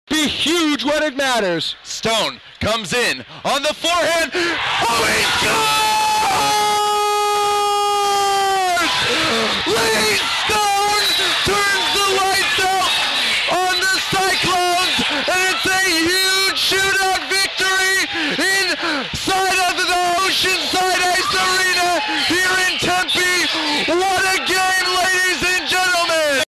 ASU beats Iowa State in Shootout
Arizona State Sundevils defenseman Lee Stone scores to win the the shootout 2-1 against the #5 ranked Iowa State Cyclones.